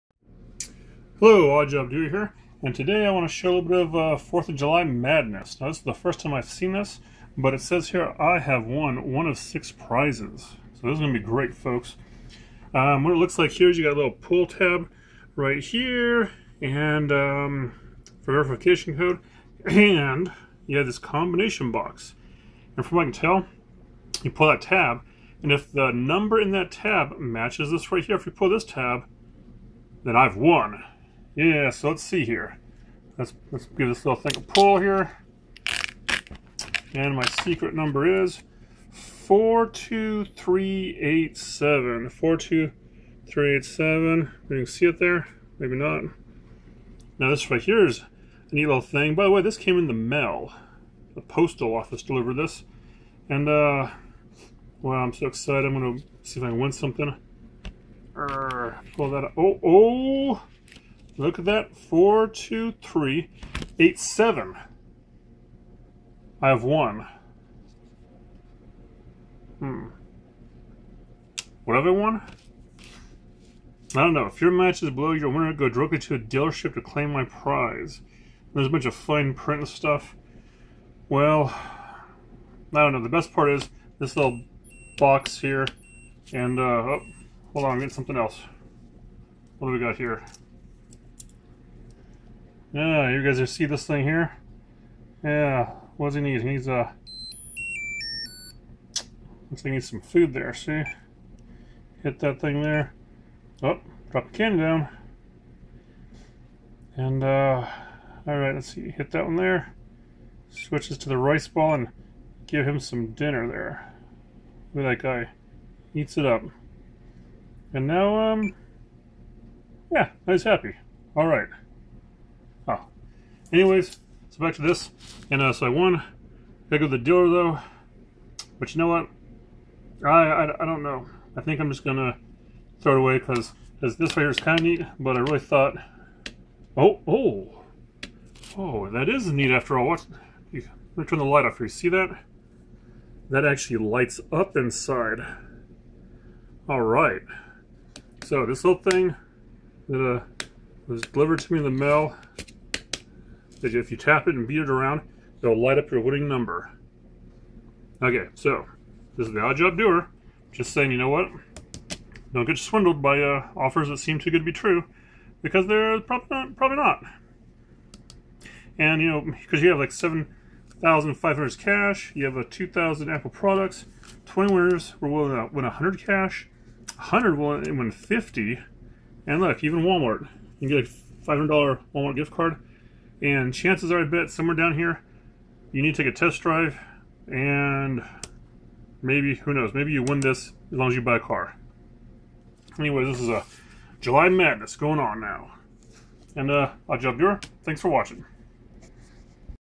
The Odd Job Do'er does a little Public Service Announcement.
The Odd Job Doer gives a little Public Service Announcement on what seems to be the best piece of mail I received all day. i am sure it is not a real scam, but i am sure it is not that great either.